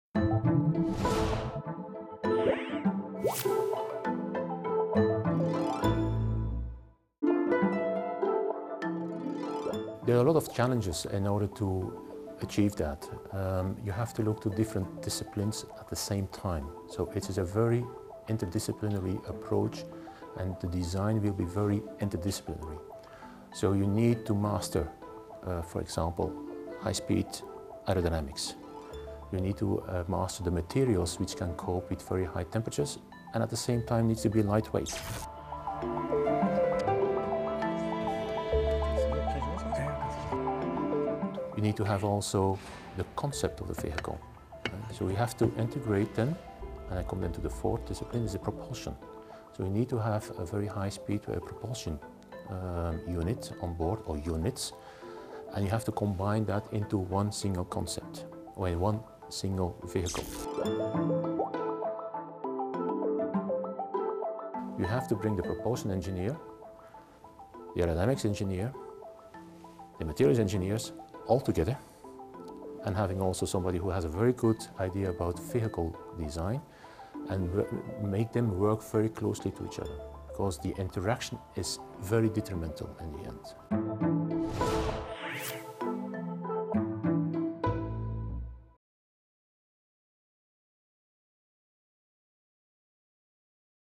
How to fly around the world in 3 hours - Do You Know?
With hypersonic jets, it can take you less time to fly from Europe to America than it takes you to get from home to the airport - but the whole industry will need to change to make that speed of travel a reality. Johan Steelant, a research engineer from the European Space Agency, tells us what to expect in the coming decades.